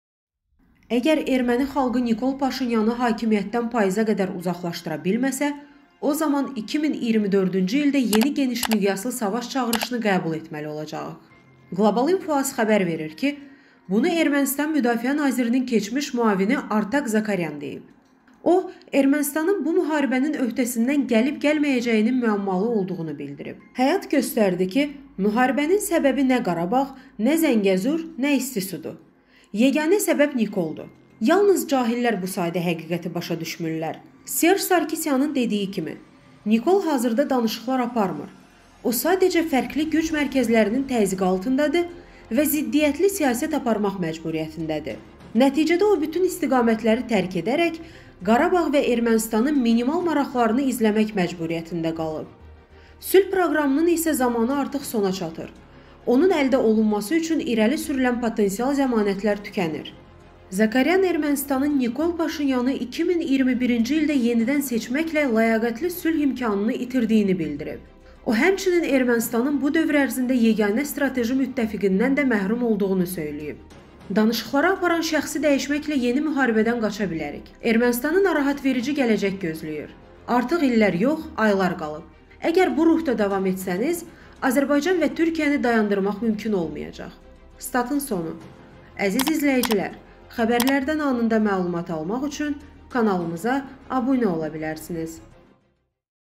Azərbaycanla genişmiqyaslı müharibə başlayacaq – Zakaryan tarixi açıqladı
Əgər erməni xalqı Nikol Paşinyanı hakimiyyətdən payıza qədər uzaqlaşdıra bilməsə, o zaman 2024-cü ildə yeni genişmiqyaslı savaş çağırışını qəbul etməli olacağıq.